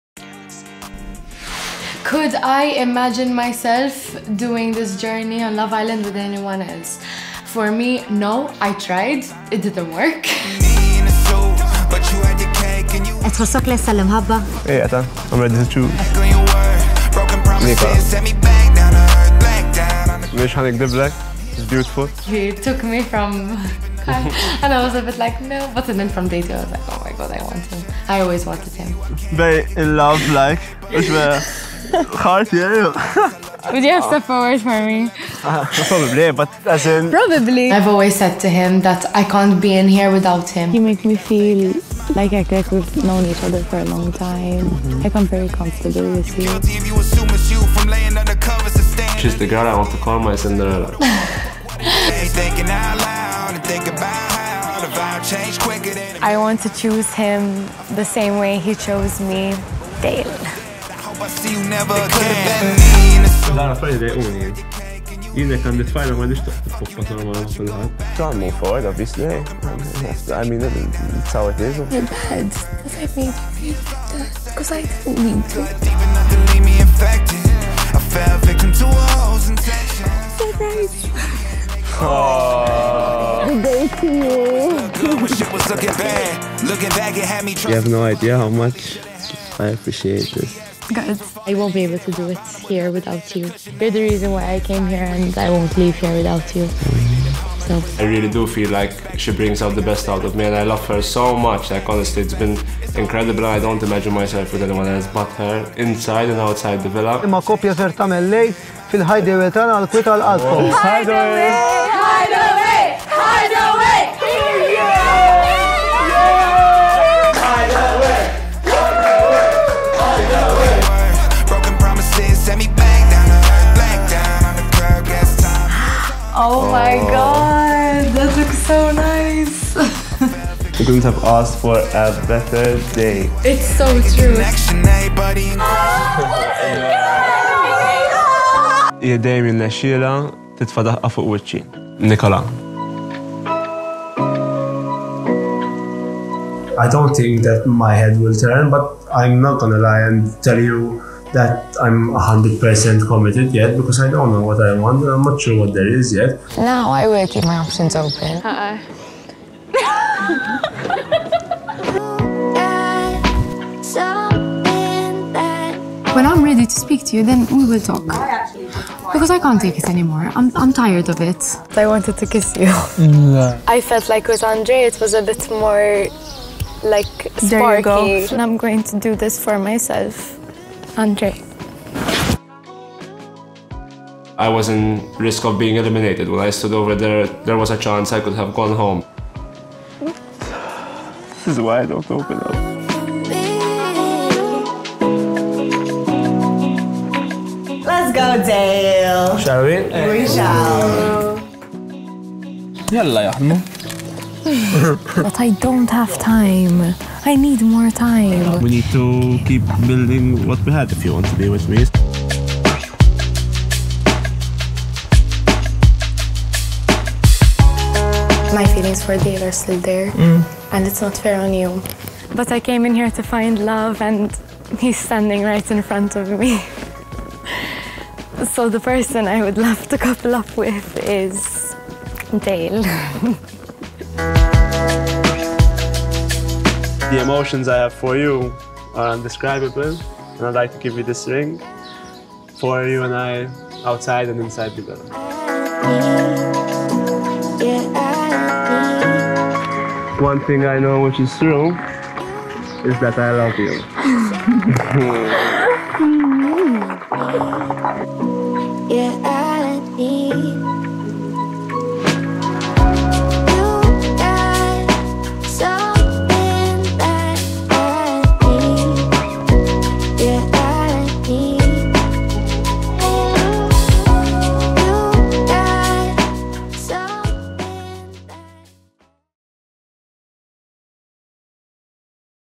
Best of Dale & Nicola
Dale and Nicola's journey has been bumpy,  but all good things are worth fighting for 💞